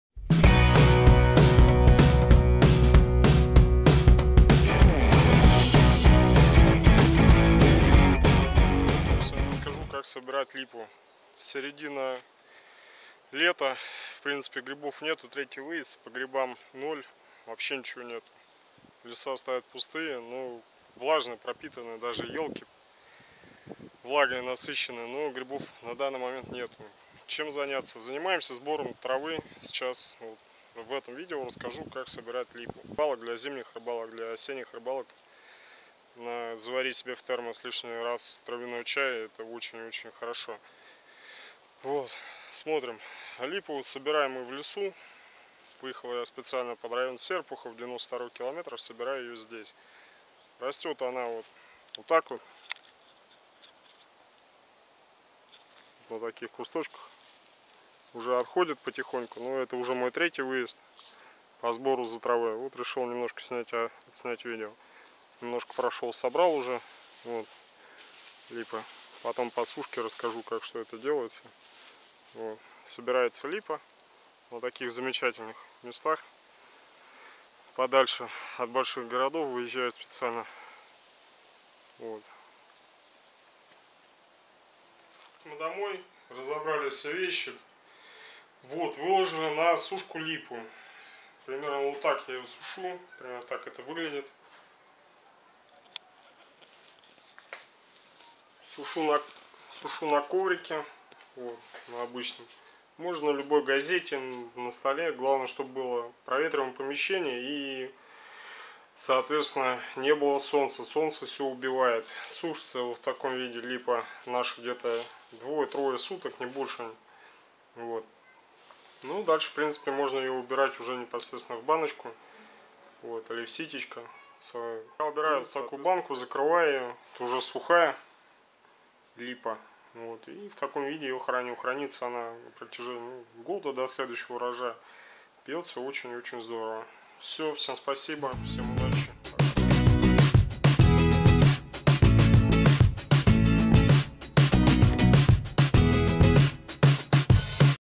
Сбор и сушка липового цвета.
Липовый чай, полезные свойства которого известны еще со времен наших прабабушек, является эффективным народным средством для лечения многих недугов.

Говоря о пользе липового чая, его можно пить, начиная от самых простых проблем со здоровьем и заканчивая самыми сложными.